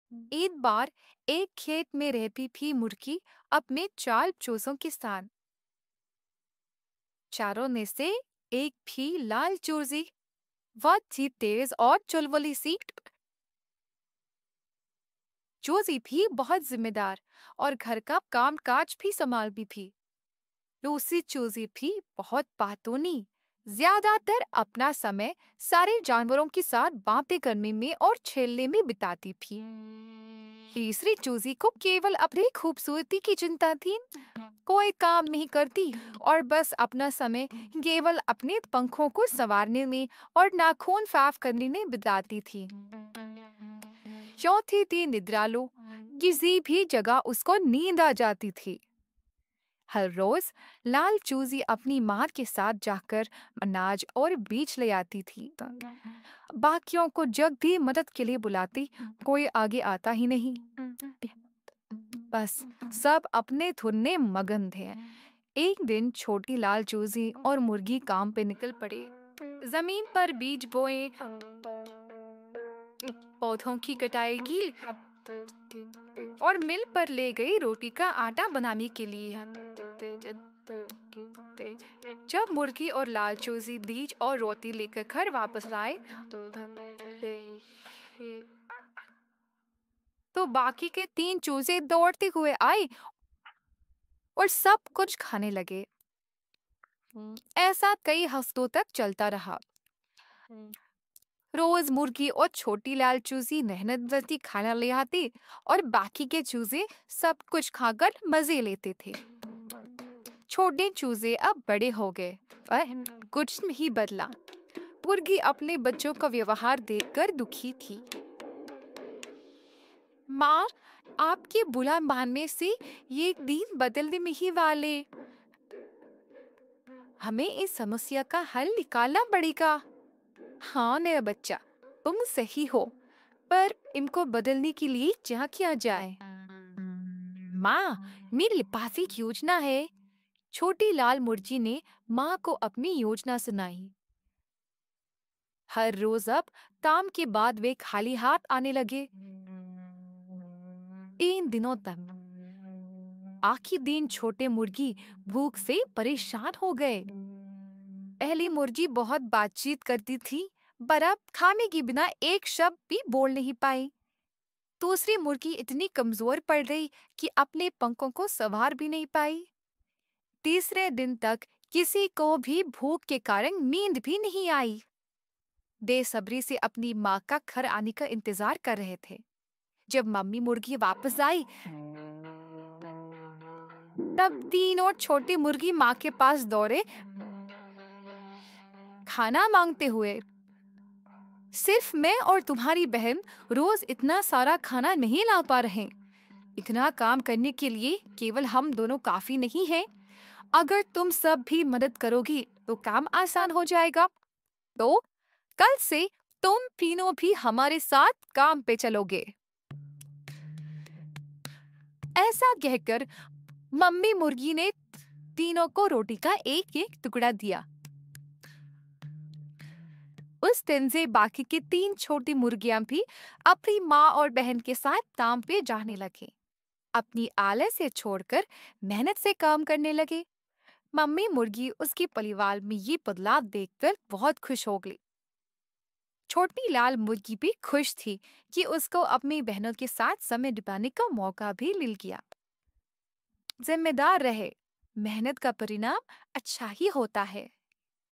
चालाक लाल मुर्गी की कहानी | Hindi Kahaniya | बच्चों के लिए प्रेरणादायक कहानी | BTTV Kids
इस वीडियो में जानिए चालाक लाल मुर्गी की मजेदार और शिक्षाप्रद कहानी। यह कहानी बच्चों को सिखाती है कि समझदारी और मेहनत से किसी भी समस्या का हल निकाला जा सकता है। इस दिलचस्प हिंदी कहानी के साथ बच्चों को नैतिकता और जिम्मेदारी का पाठ सिखाएं।